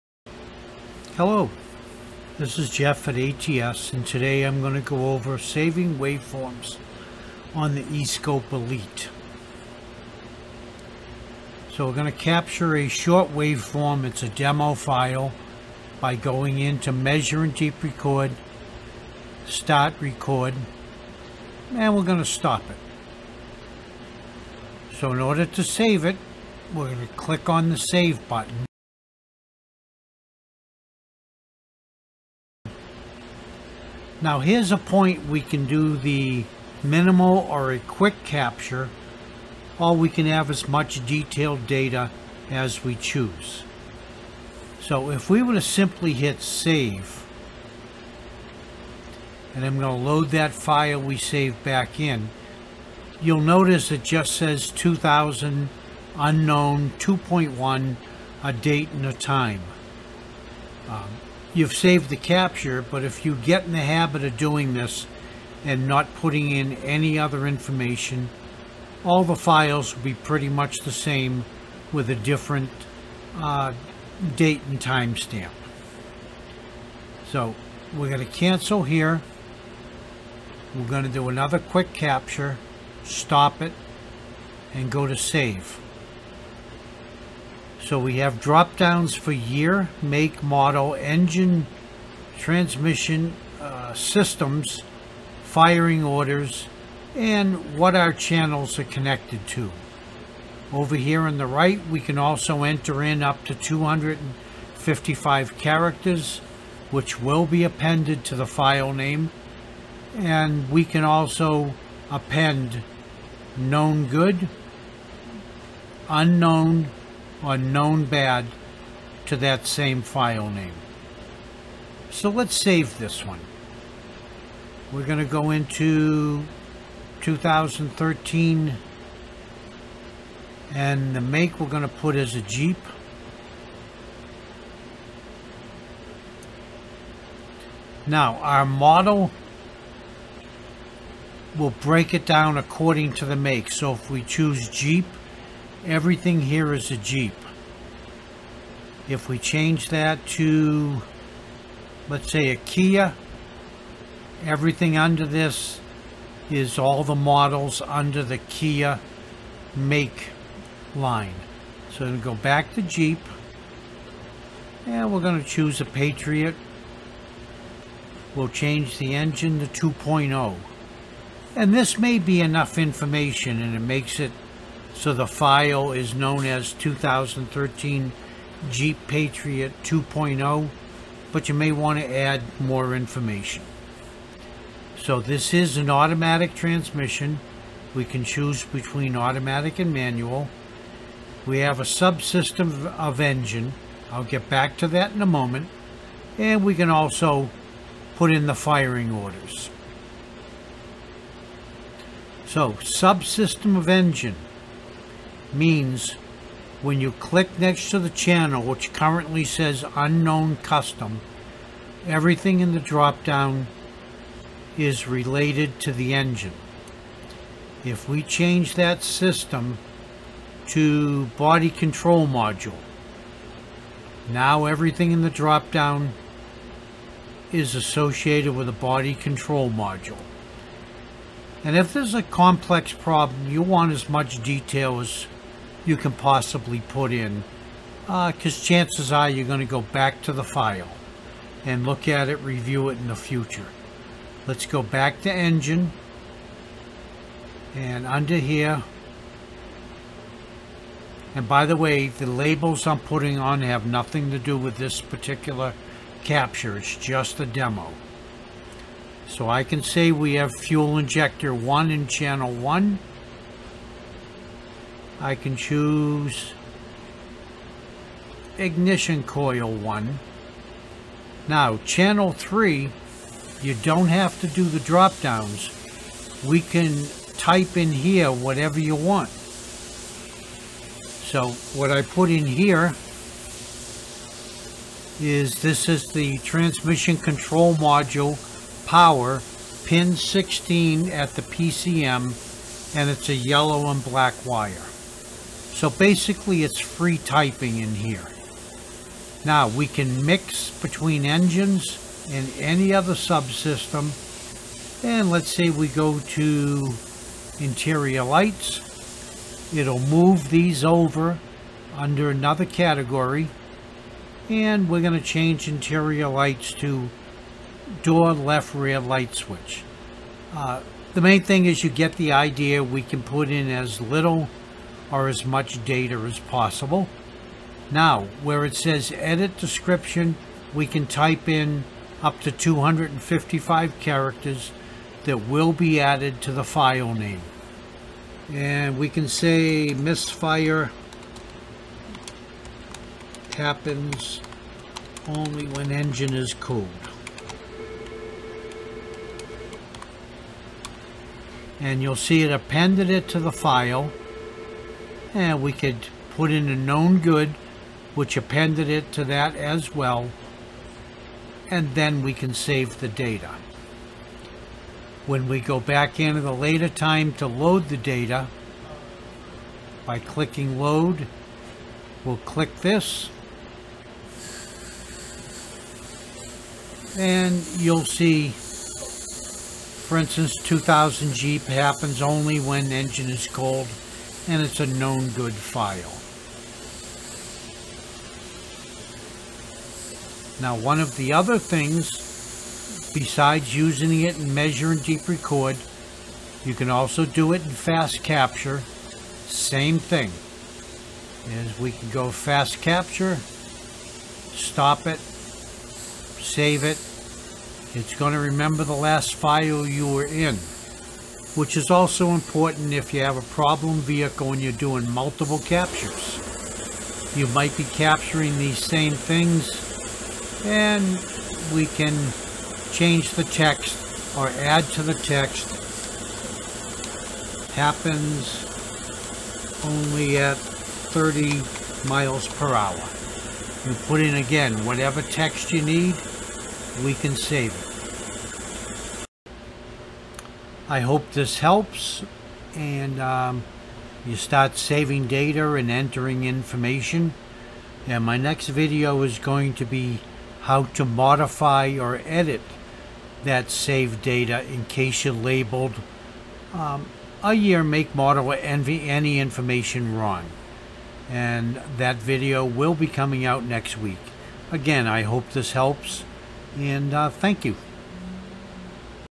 Saving Waveforms in the eSCOPE ELITE
How to Save and Name Waveforms on the ATS eSCOPE ELITE